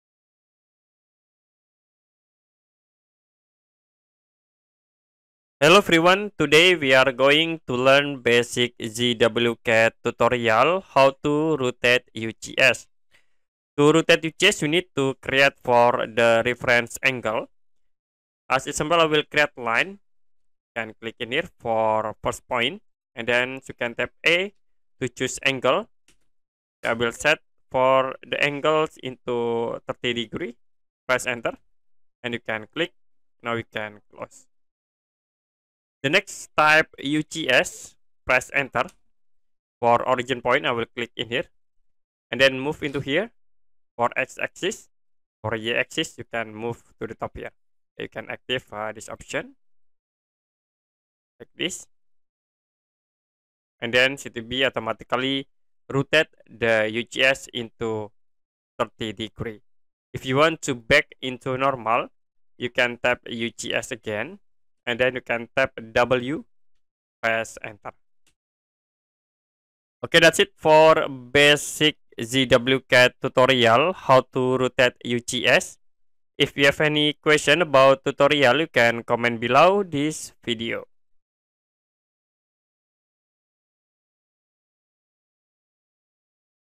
How To Rotate UCS In ZWCAD
Learn how to rotate the User Coordinate System (UCS) in ZWCAD with easy step-by-step instructions. This guide will help you understand the process and master this feature in ZWCAD.